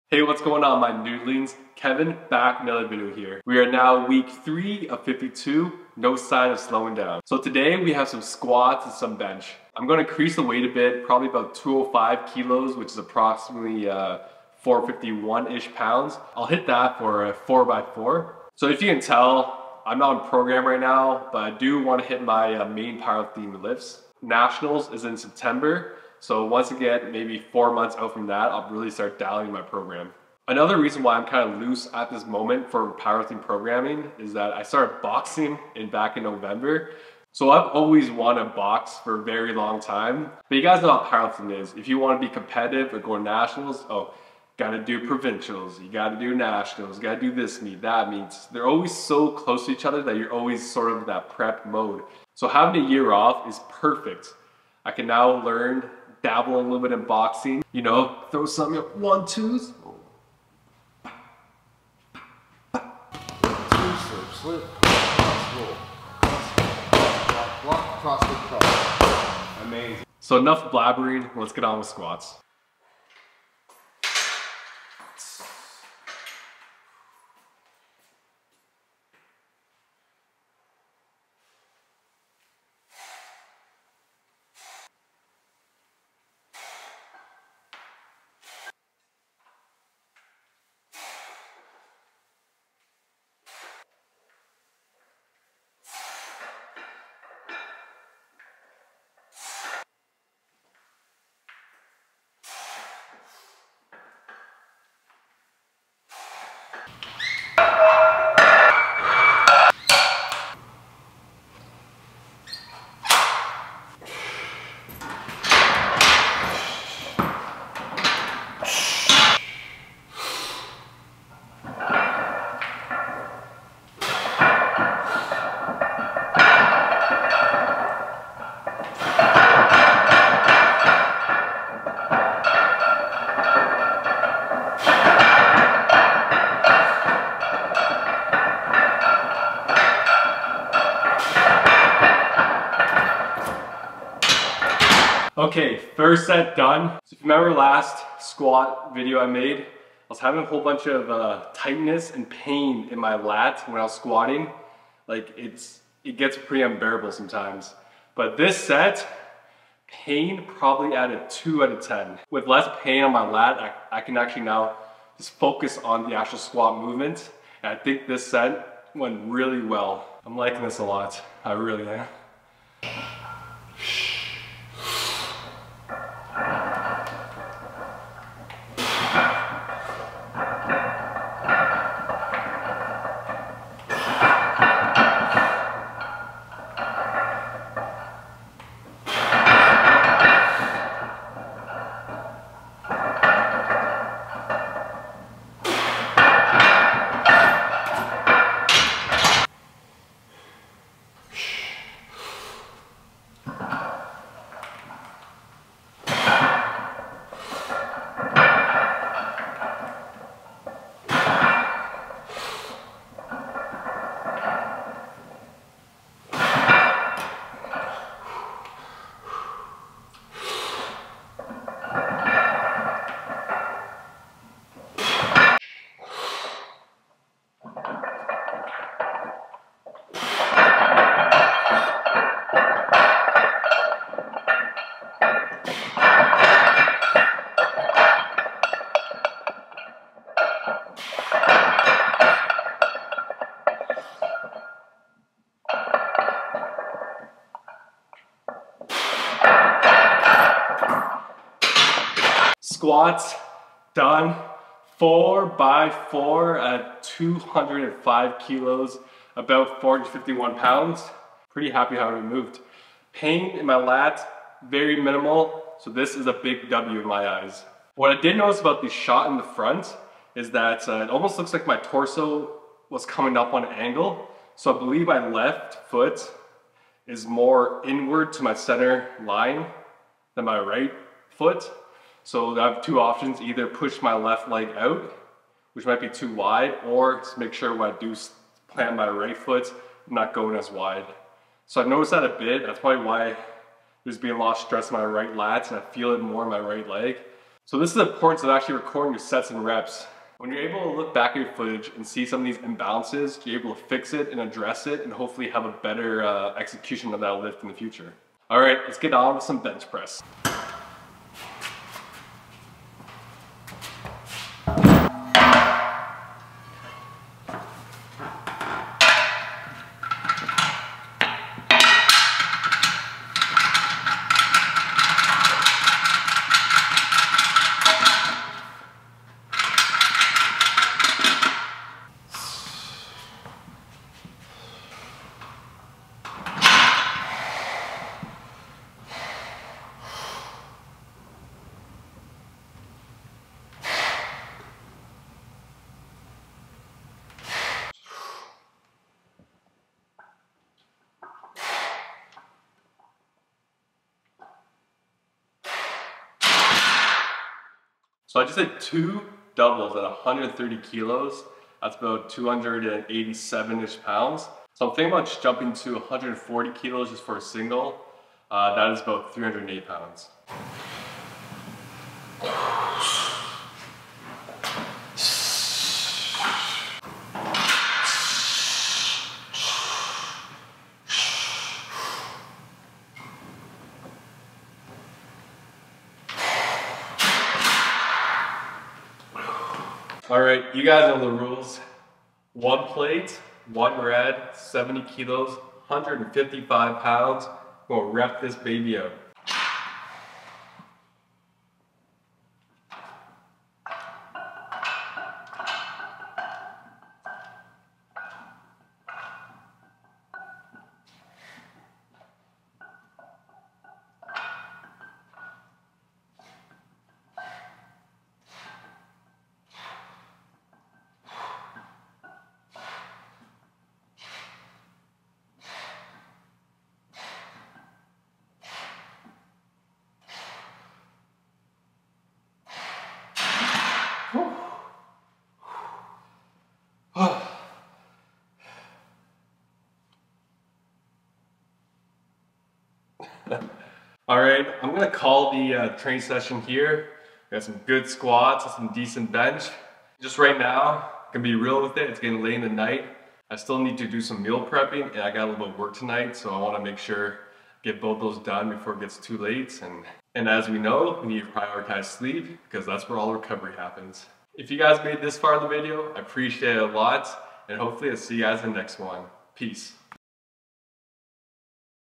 Squats Are Getting Better !
Squats are feeling better in terms of less pain in the lats when I'm squatting. That means I can focus on squats rather than worrying about the lats!

My Media Setup:

Camera: Sony A7iii
Lense: Sony G 20mm F.18
Microphone: Rode Videomic Pro